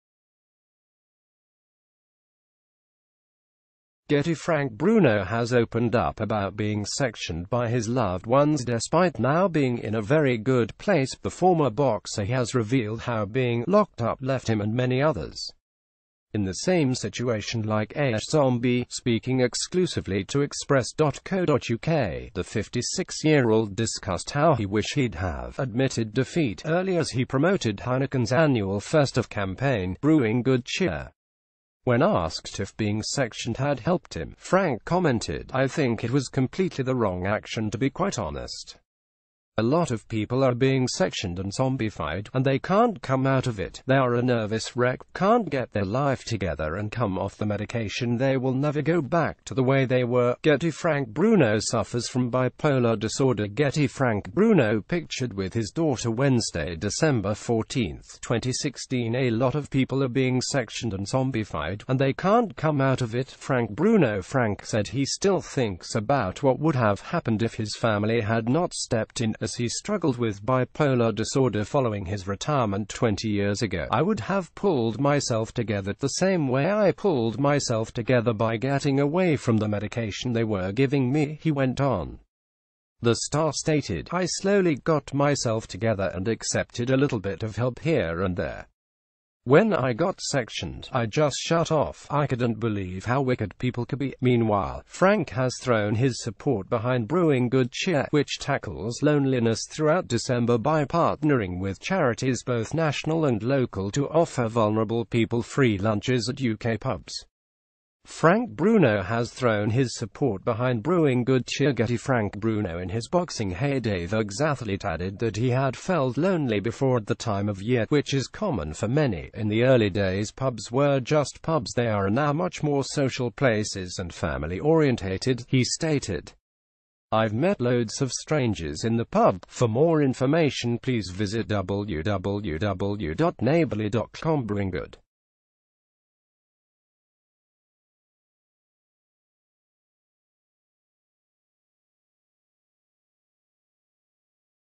Frank bruno, 56, admits being sectioned was ‘completely the wrong action’ by his family| Breaking N
Frank bruno, 56, admits being sectioned was ‘completely the wrong action’ by his family| Breaking News TV
GETTYFrank Bruno has opened up about being sectioned by his loved onesDespite now being in a very good place, the former boxer has revealed how being ...